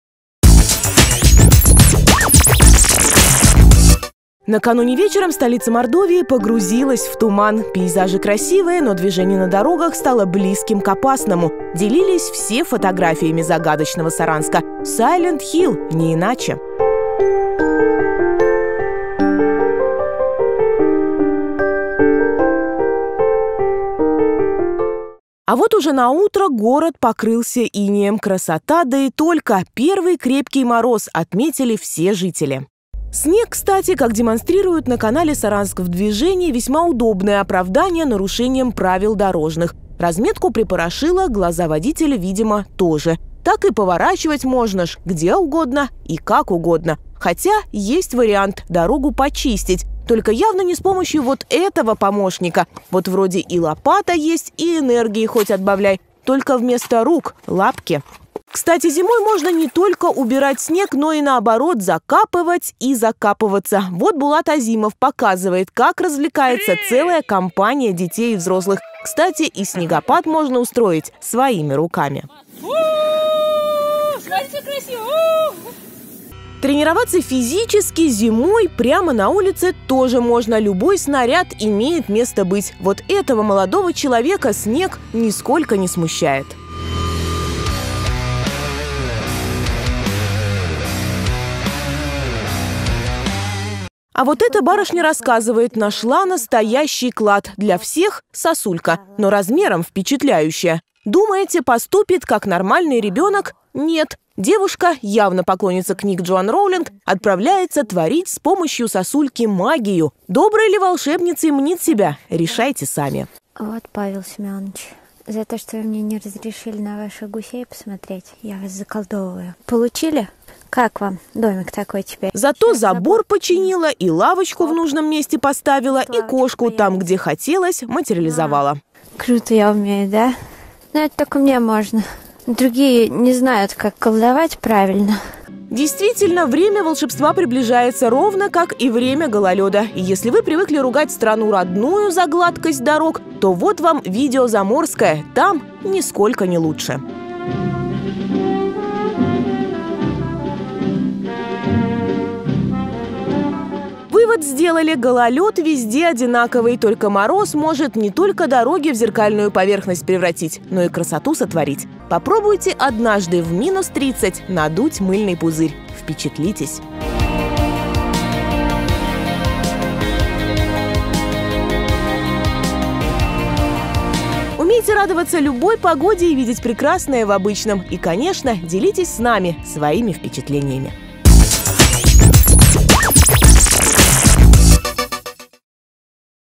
Интернет-обзор за 28 ноября 2018 года
Настало время ваших новостей в Наших Новостях. Главное, о чем говорите в сети Интернет – зима подкралась совсем близко. Плюсы и минусы похолодания, и чему можно порадоваться с наступлением  морозов – в нашем сегодняшнем Интернет-обзоре.